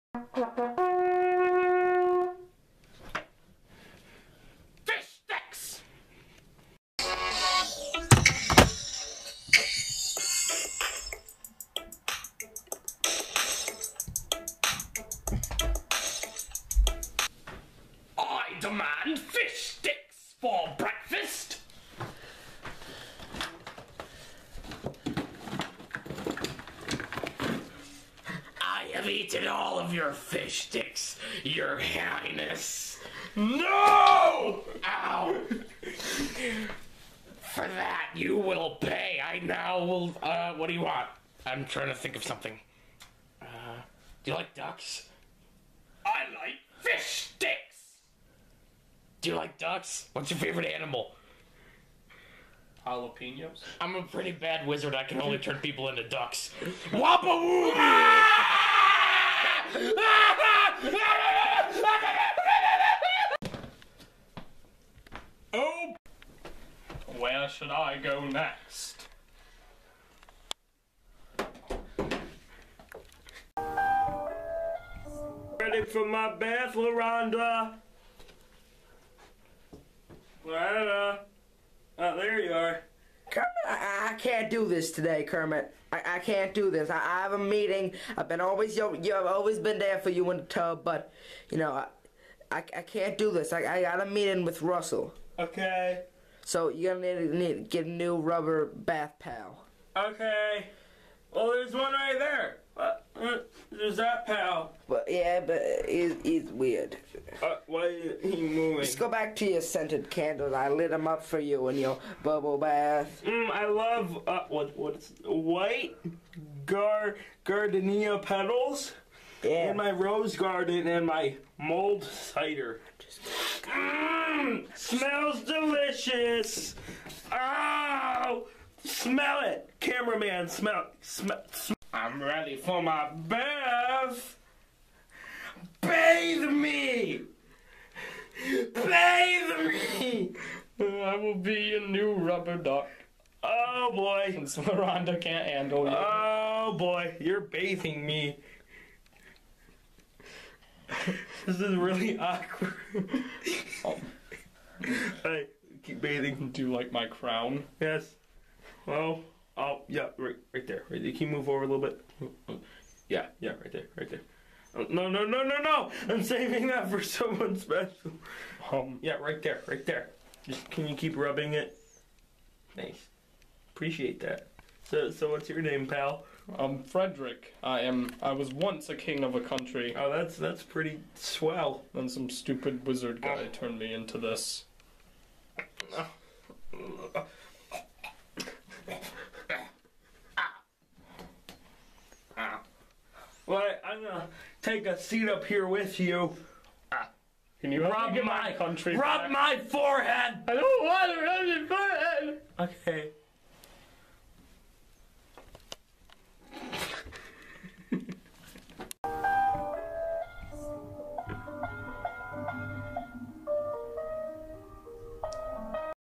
Kermits Bath Time
King Fredrick Becomes Kermit's new bath pal.

KPV is back
Weekly videos on Friday
More Sunny-D then Ever